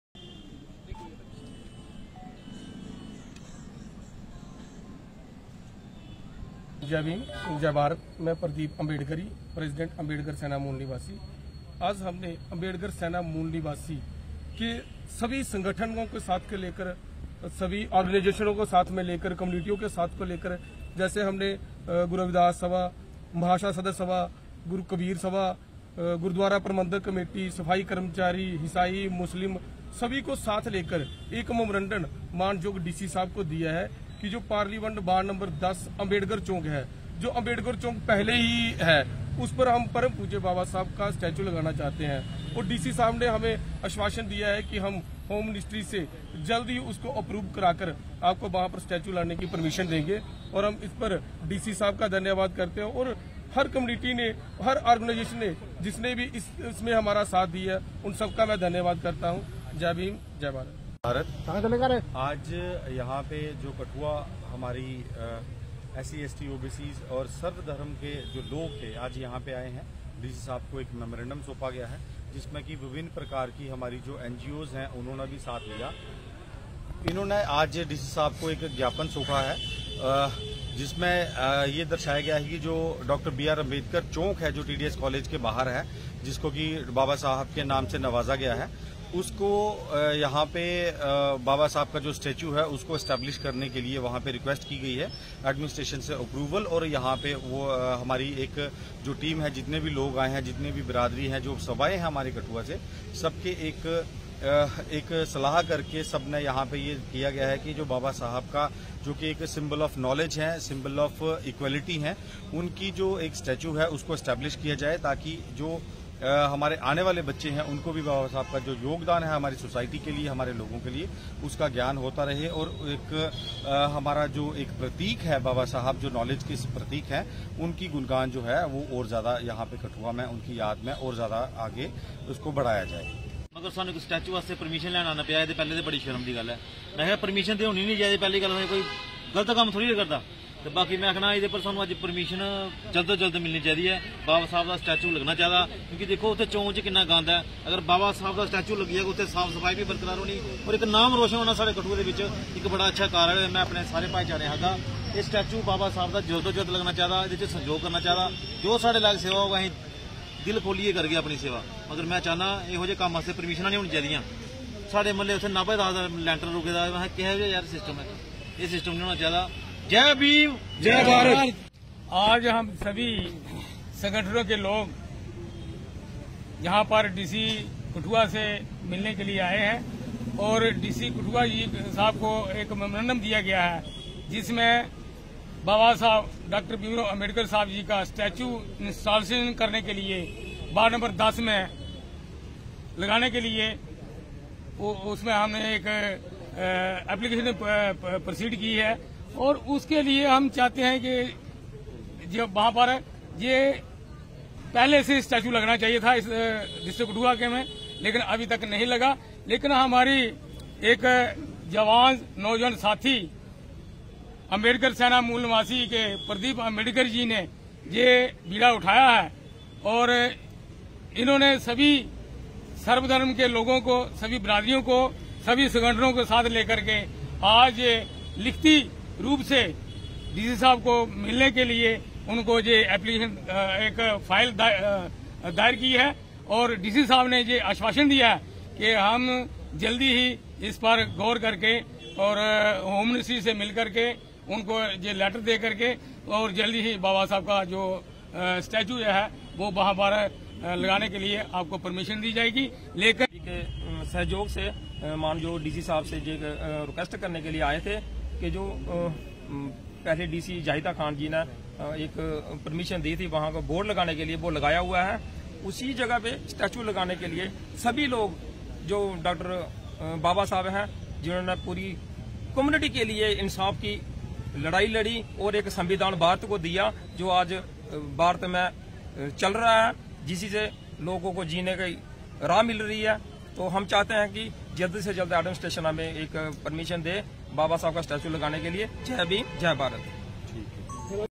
डॉ भीमराव अंबेडकर जी की प्रतिमा वार्ड नंबर 10 कठुआ में स्थापित करने की मांग को लेकर डीसी कठुआ।
डॉ भीमराव अंबेडकर जी की प्रतिमा वार्ड नंबर 10 कठुआ में स्थापित करने की मांग को लेकर डीसी कठुआ से मिलने पहुंचा प्रदीप अंबेडकर की अध्यक्षता में शिष्ट मंडल।